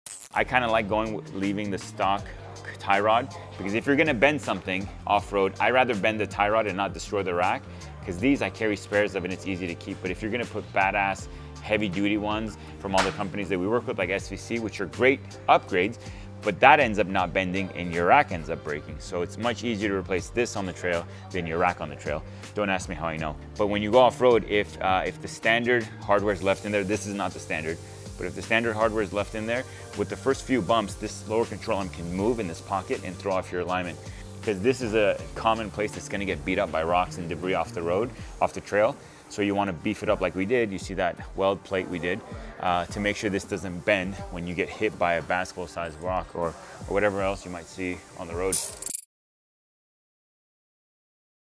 Best Ford Raptor Advice You'll Ever Hear!
OEM vs. aftermarket tie rods? Here's every reason why we suggest leaving the factory tie rods in place on a modified, First-Gen Ford Raptor!